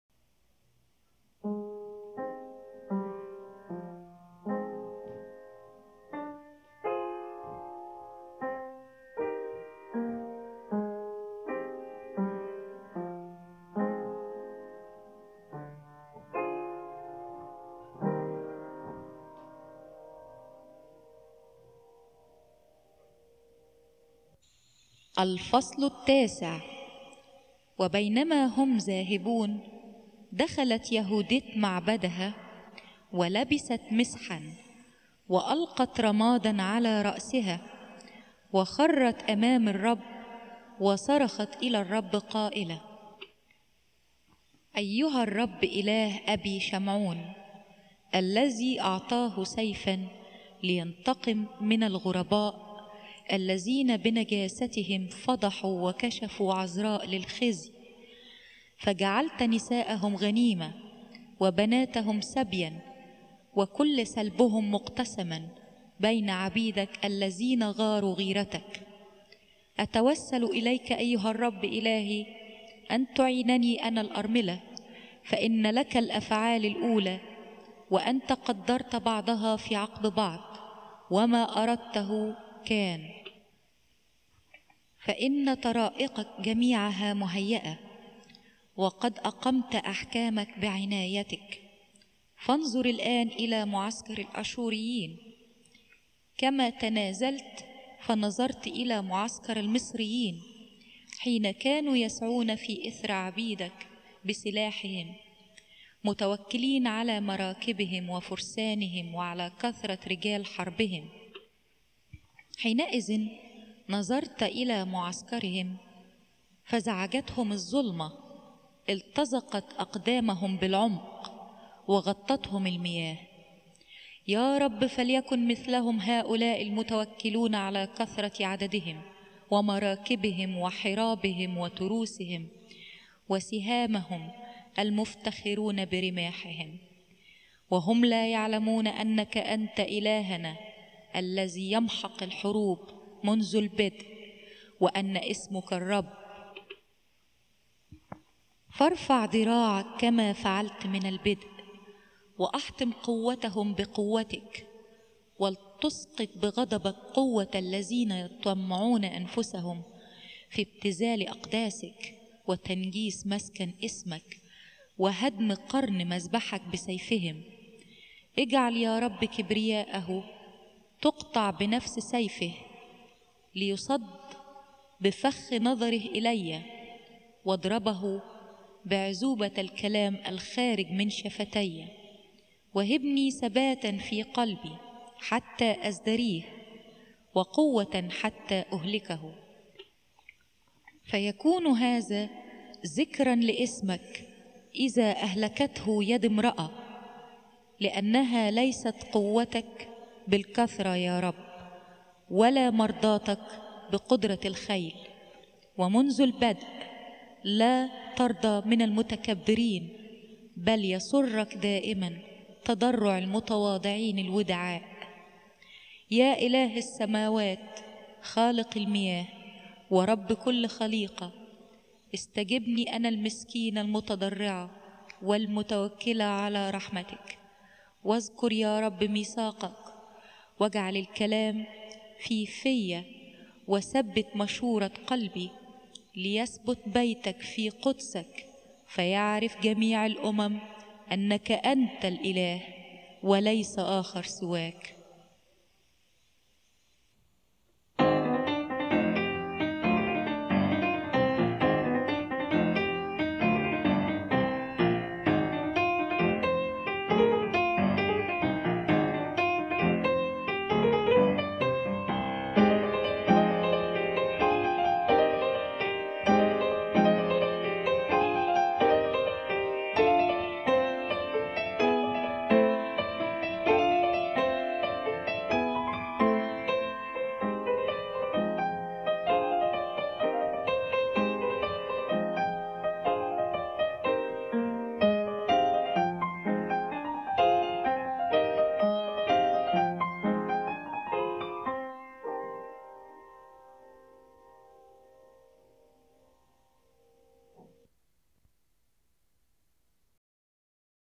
Judith (Arabic) Chapter 09 - سفر يهوديت
الكتاب المقدس
الأسفار القانونية الثانية
سفر يهوديت

Deuterocanonical Bools
The Book of Judith

Arabic narration by Mary Fass
Incidental music by Jim Fass